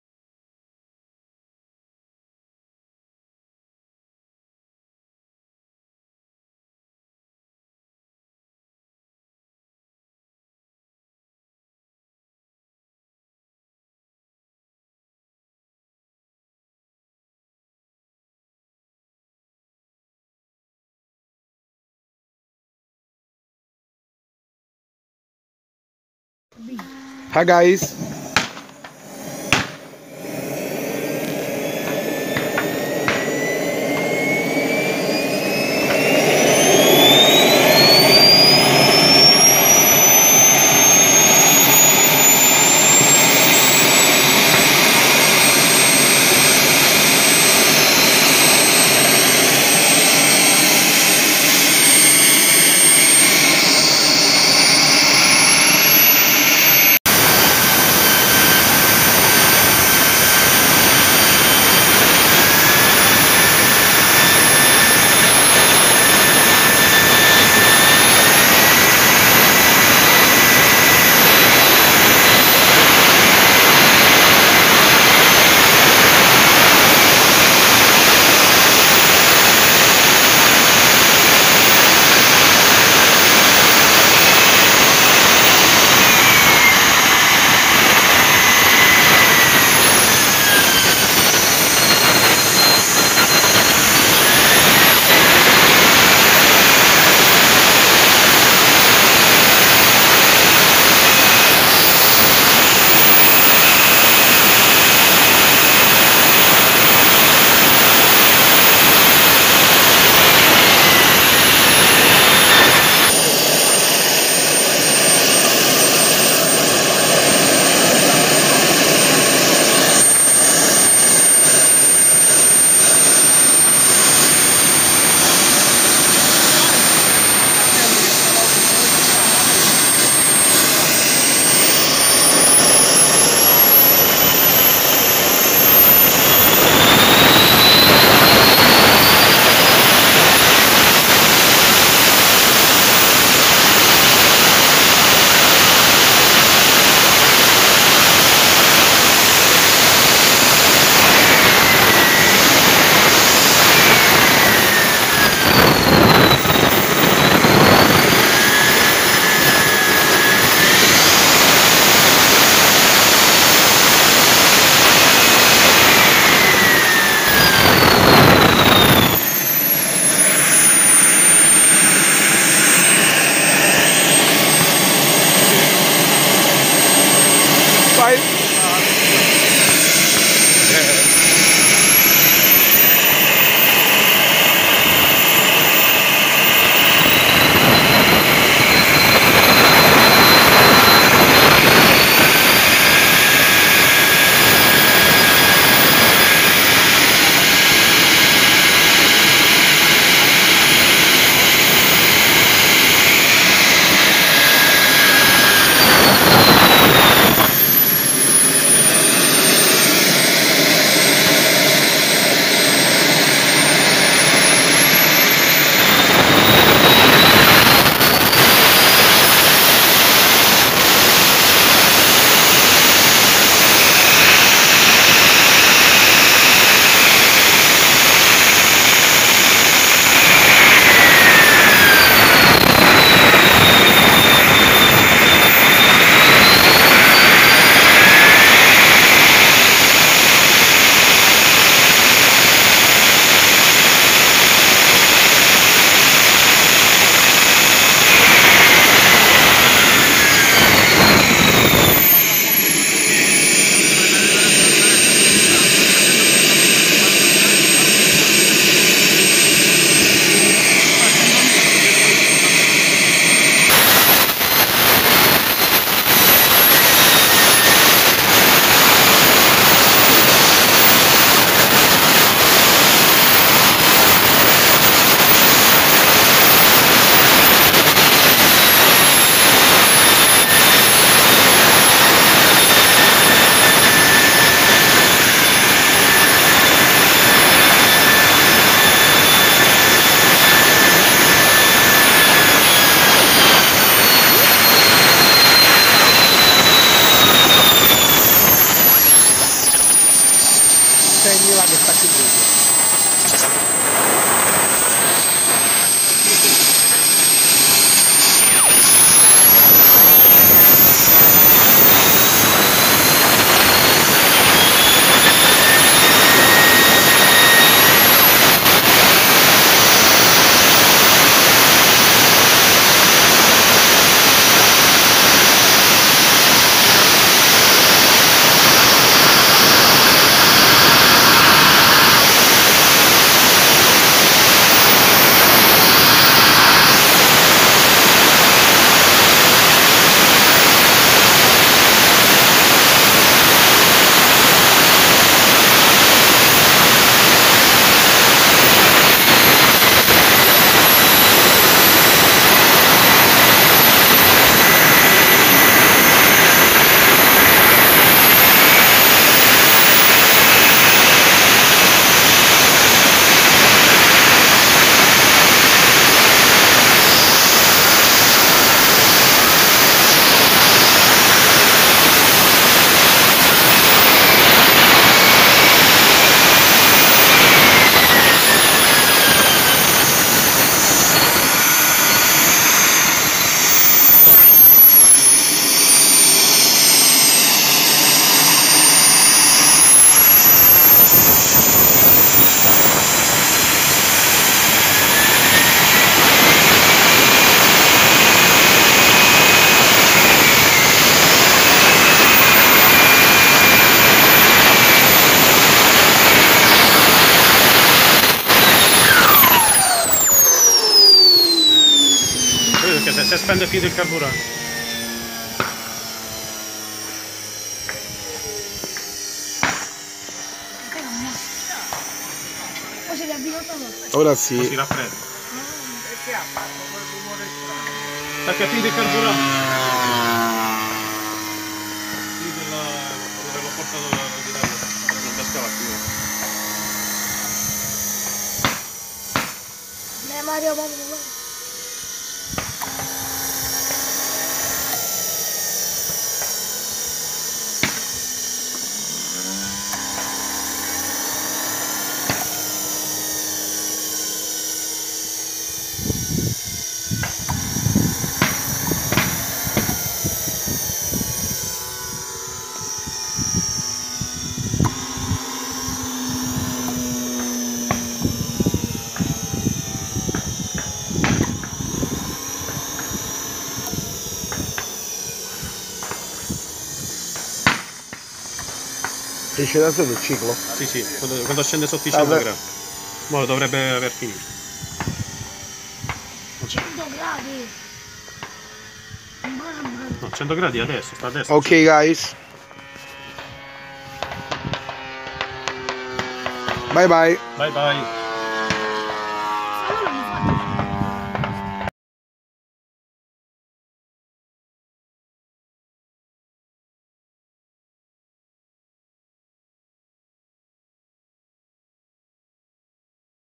Modellismo AEREO RC - collaudo TURBINA per aeromodellismo a basso costo - La JetJoe 1400
Modellismo AEREO RC - collaudo TURBINA per aeromodellismo a basso costo - La JetJoe 1400 - First test RC Jet Engine JetJoe 1400: the best low-cost turbine.

Turbine Jet Joe 1400 Thrust 6Kg at 160000 rpm gas starting and automatic passage to kero. Fully assembled by Hobby Technology